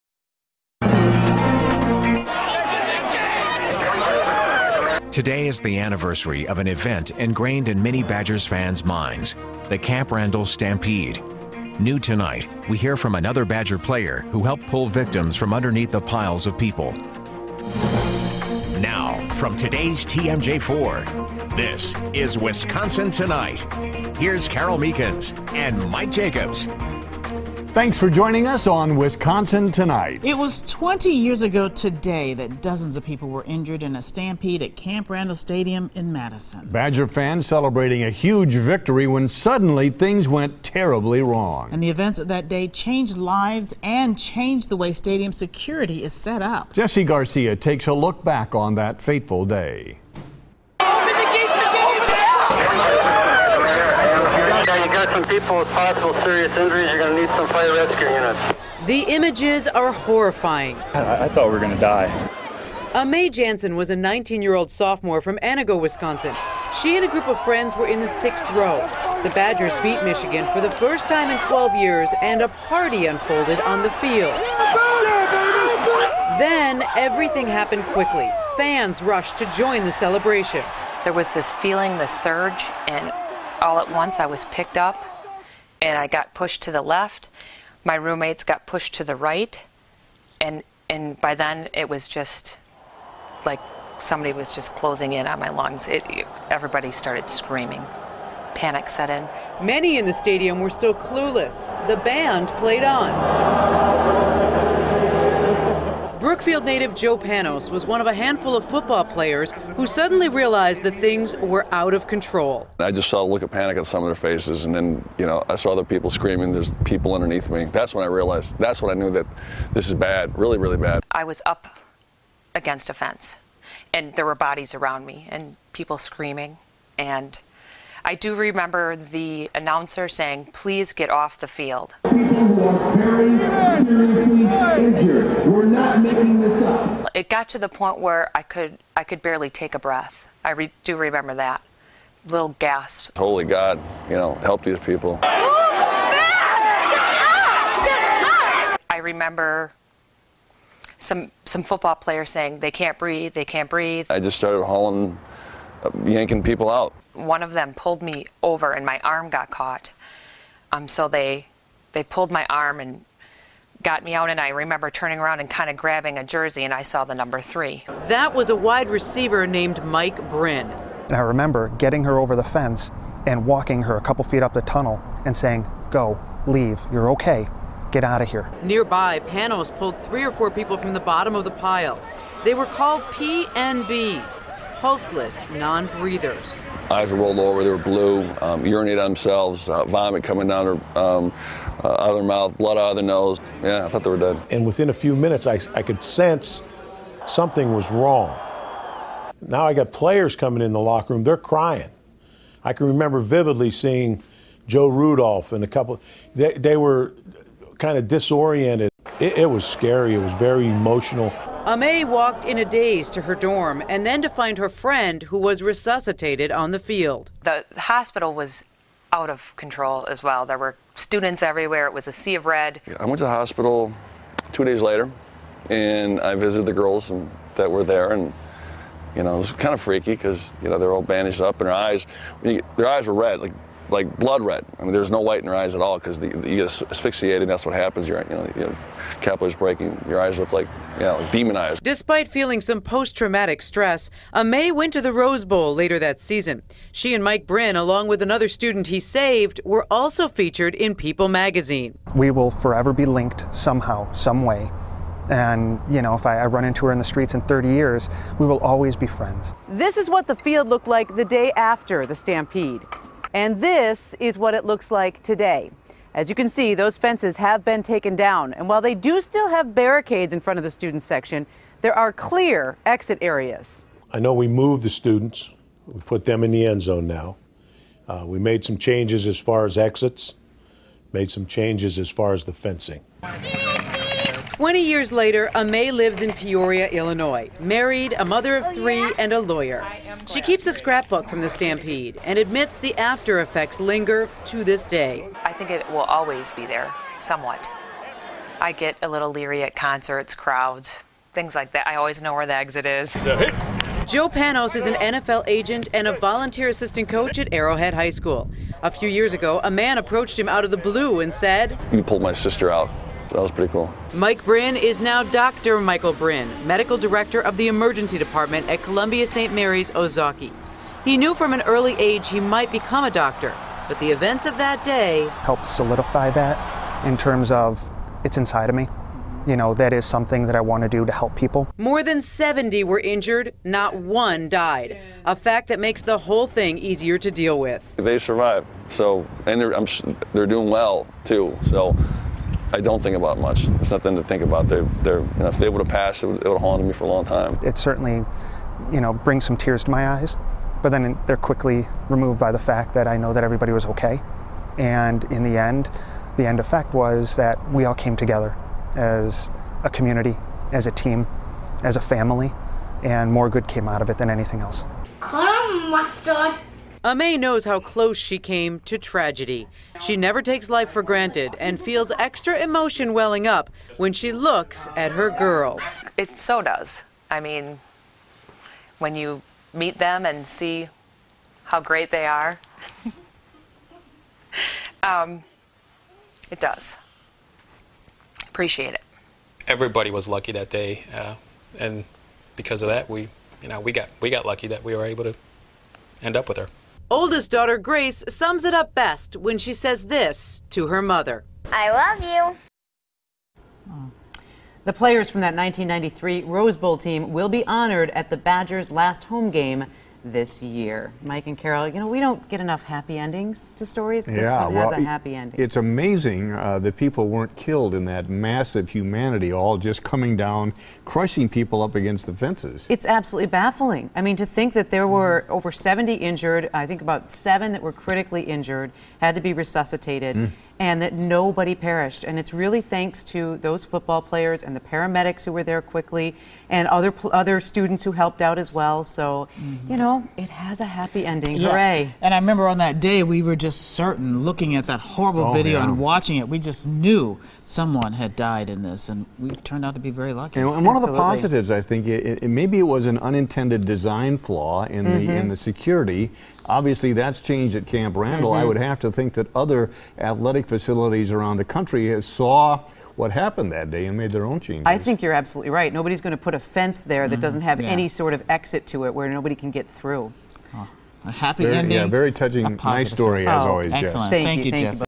Camp Randall stampede
Jessie Garcia speaks to some of the people involved in the Camp Randall stampede back in 1993.